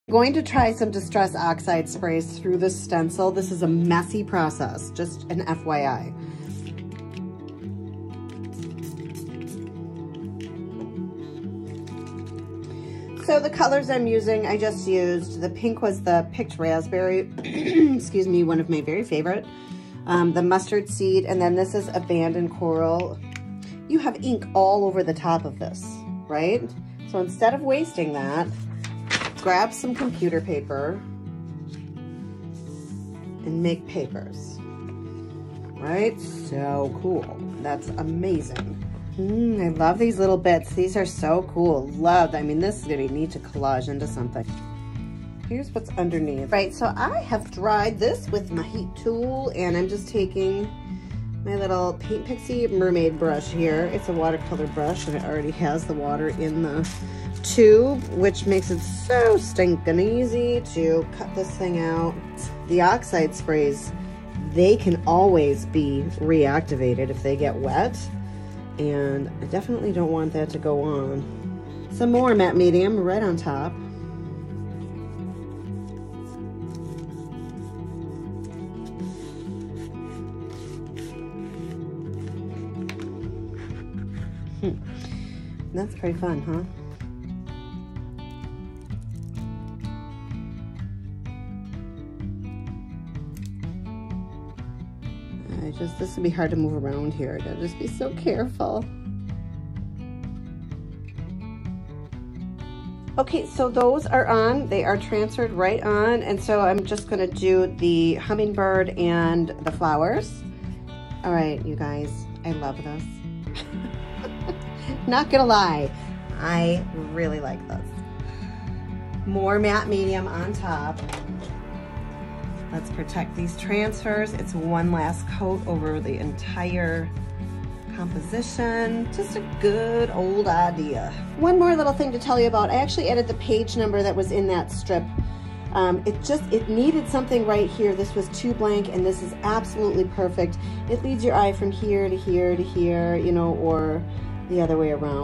DIY Mixed Media Bird Collage using Stenciling, Decoupage and Transfers
For this beautiful collage, I used a combination of techniques:
creating handmade paper with stencils & distressed oxide sprays,
decoupaging

and applying transfers.

What do you think of the finished collage?